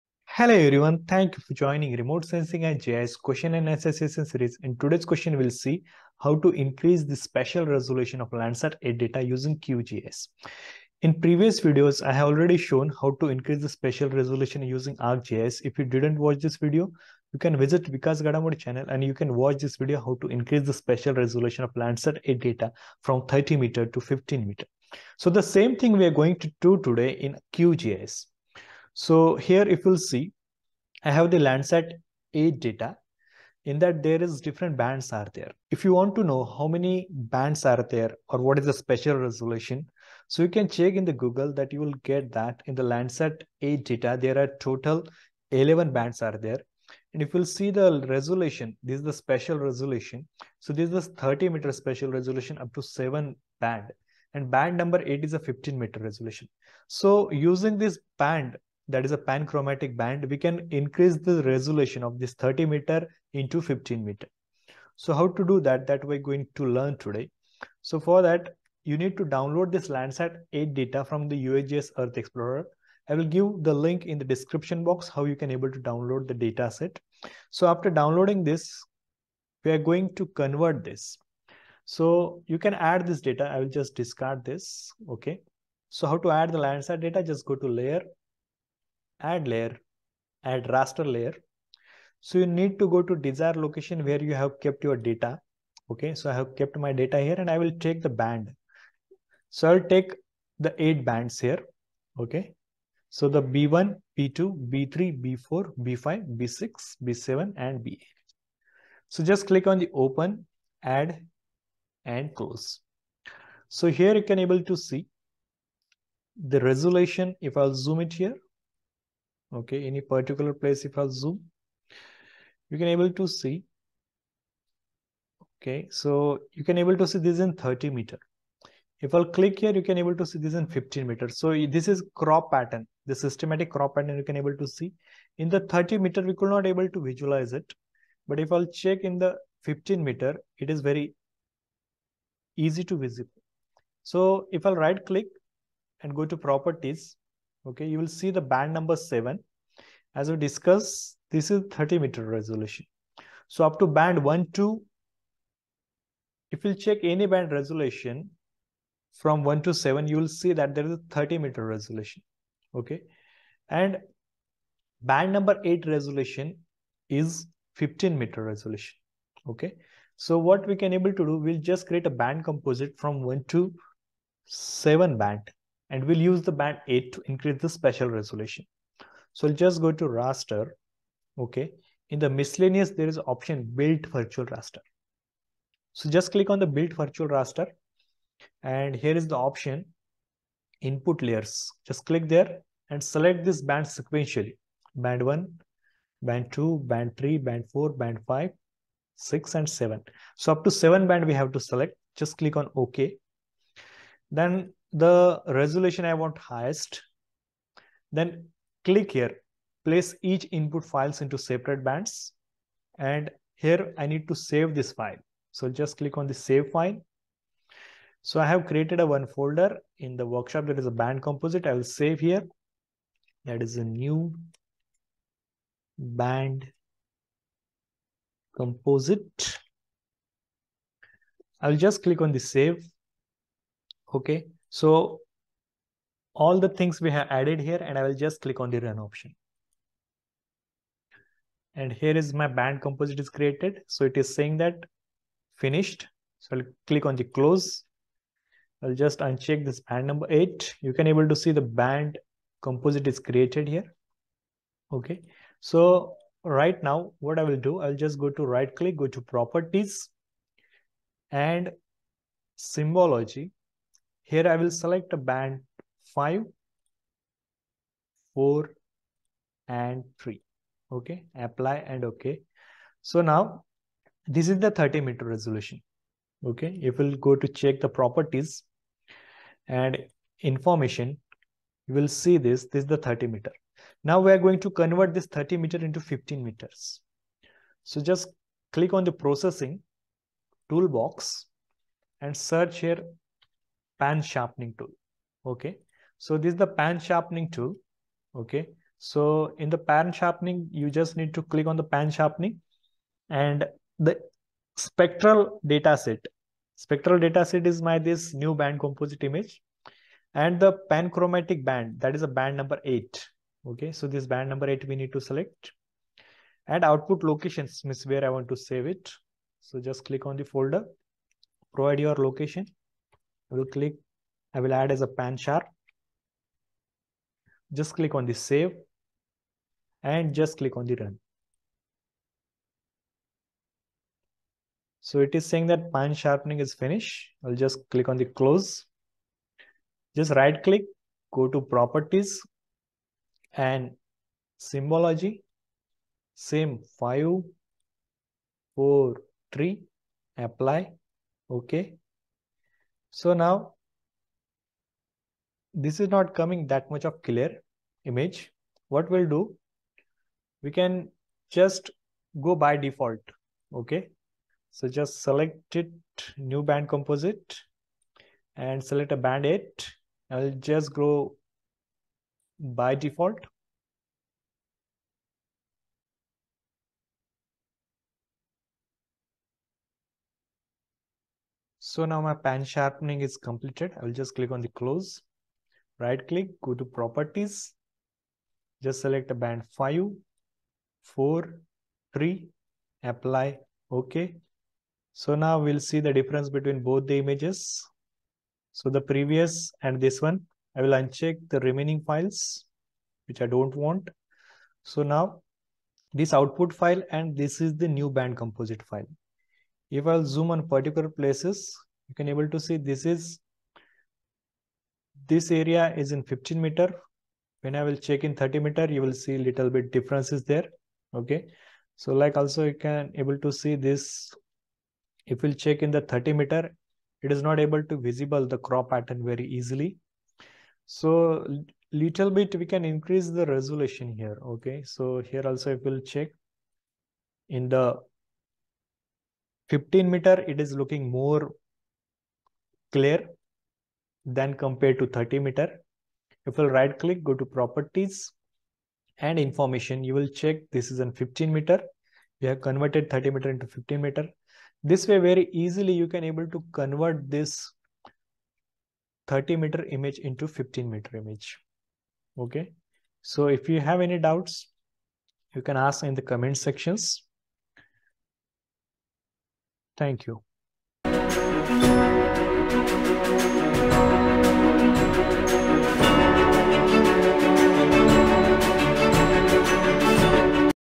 RS & GIS QA32 How to Change Spatial Resolution (30m to 15m) of Landsat 8 Data using QGIS
How to Change Spatial Resolution of Landsat 8 Data using QGIS

In QGIS, you can use the "Pansharpening" tool to change the spatial resolution of Landsat 8 data. The pansharpening tool allows you to combine high-resolution panchromatic data with lower resolution multispectral data to produce a high-resolution multispectral image.

Resampling changes the resolution of a single image, while Pansharpening combines two different images with different resolutions to create a new image with improved resolution.

00:00 Intro
00:15 Increase resolution using Arcgis
00:44  Landsat 8 Bands
01:26 Add Landsat 8 Data
03:01 Built Virtual Raster
04:53 Pan sharpning tool